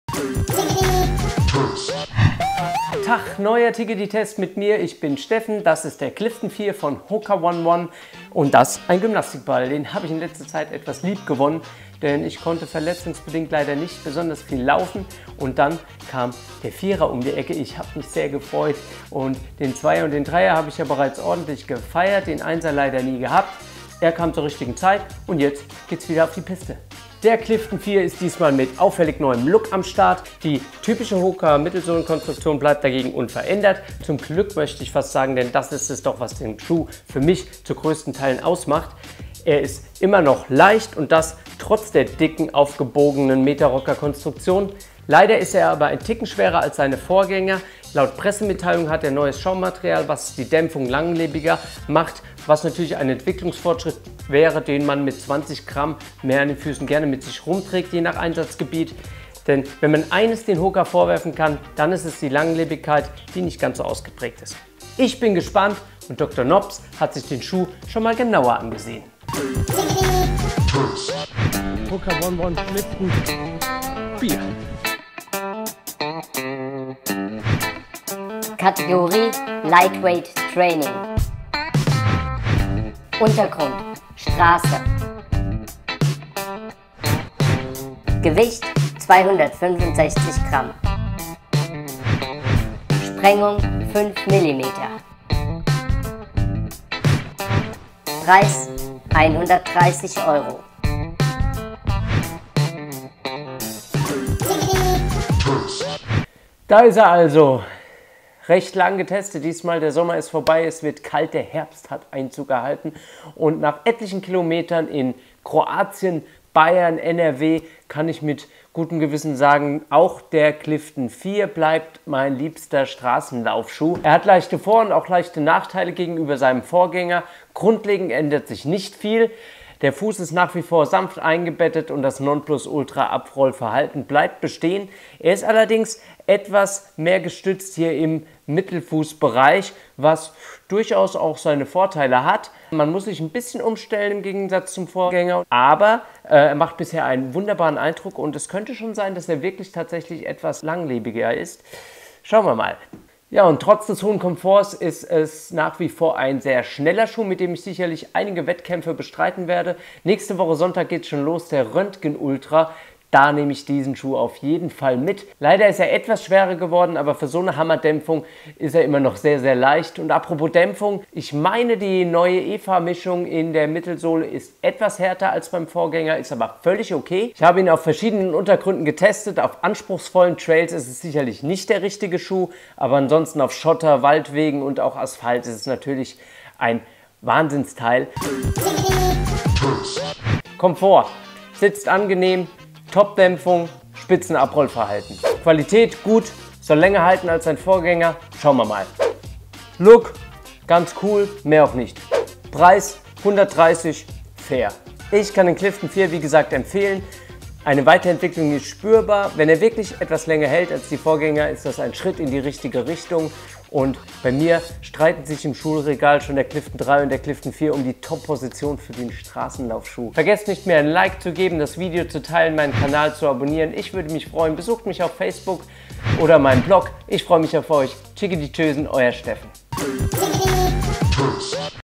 Hoka One One Clifton 4 - Tiggedi Test
Bei "Tiggedi Test" werden Sportartikel getestet.

Ich bekomme für dieses Video übrigens kein Honorar vom Hersteller.